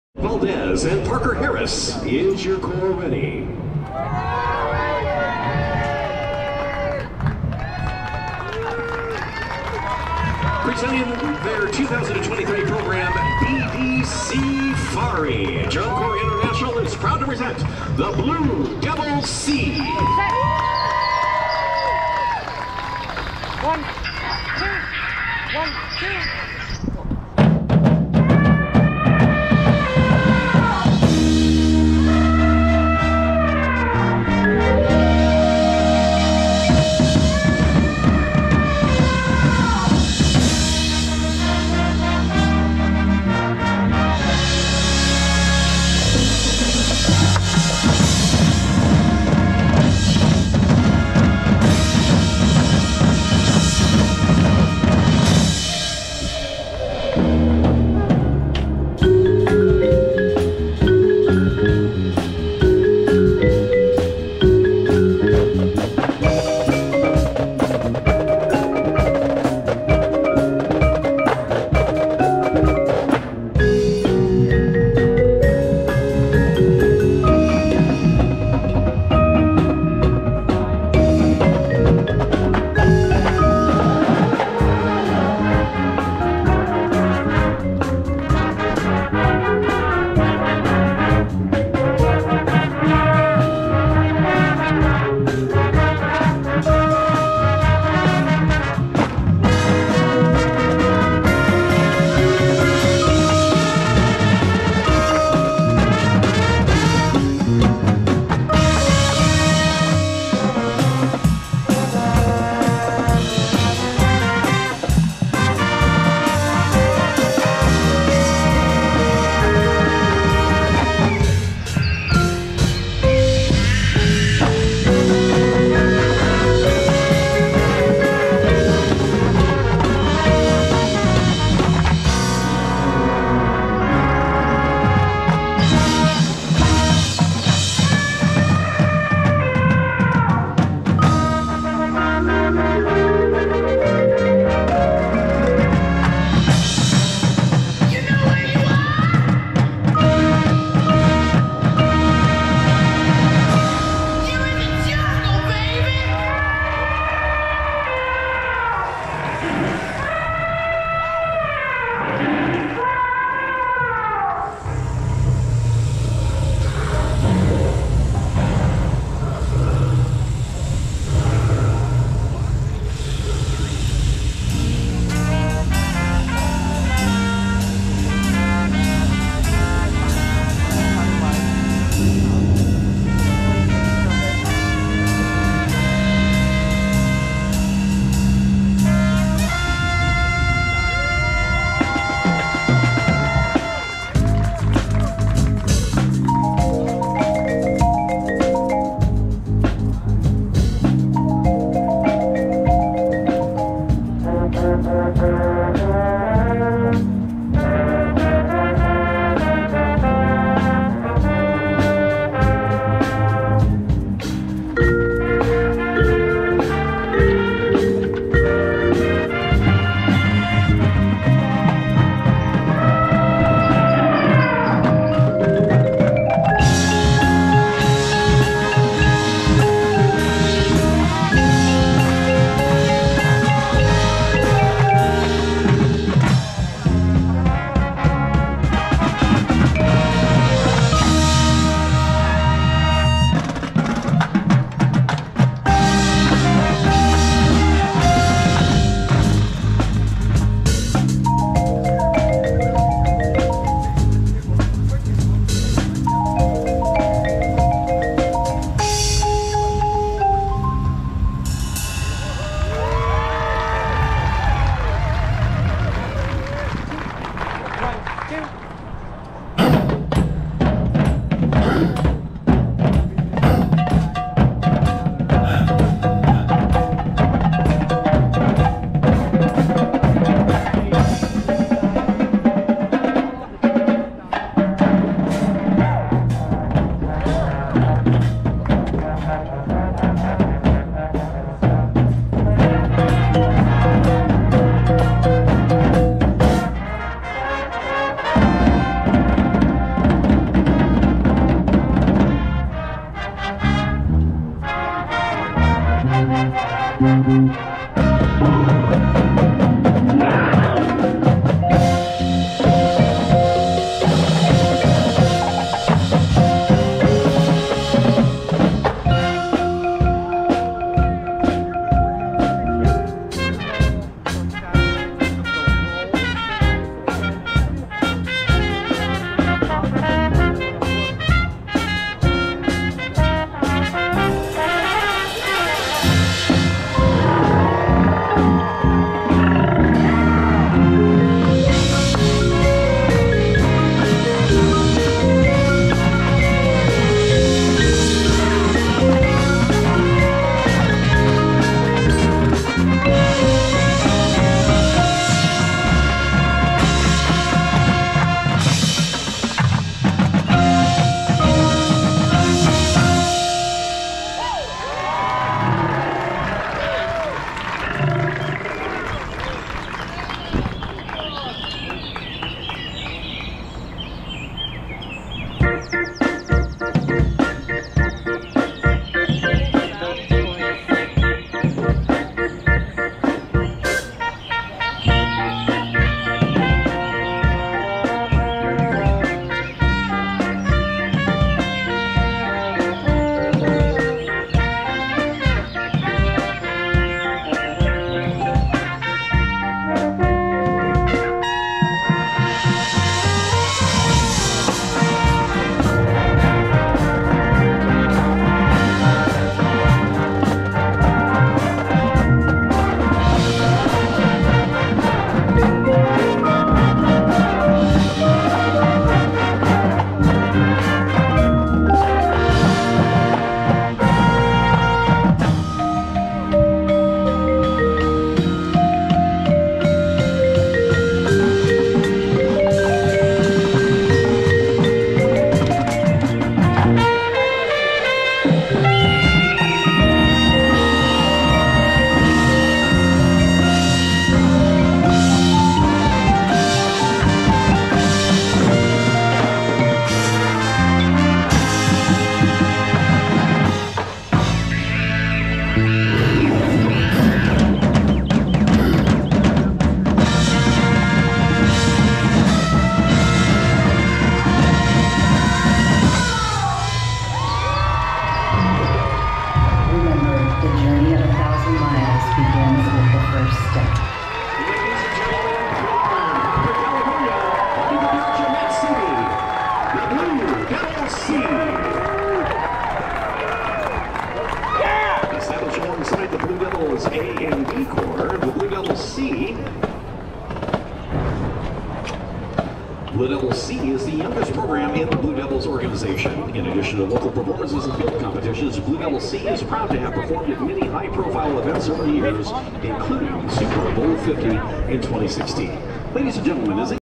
Blue Devils C Drum And Bugle Corps 2023 SoCal Classic Drum Corps International, DCI West
Blue Devils C Drum And Bugle Corps at the SoCal Classic 2023 BDC-fari bdc2023 evansdrumheads dci drumcorpsinternational todgomesphotography, 59.95